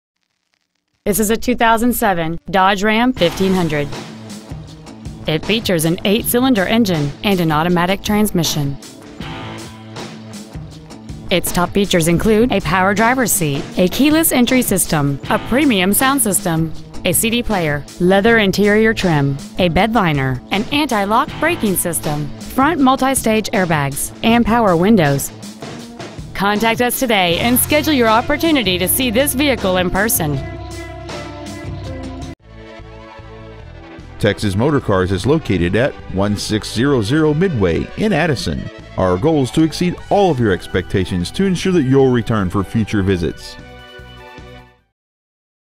Used 2007 Dodge Ram 1500 Carrolton TX
Year : 2007

 Make : Dodge

 Trans . : Automatic

 Exterior : White

 Miles : 78,338

 Interior : Gray

 Stock : 131071

 Texas Motor Cars

 1600 Midway

 Used 2007 Dodge Ram 1500  TX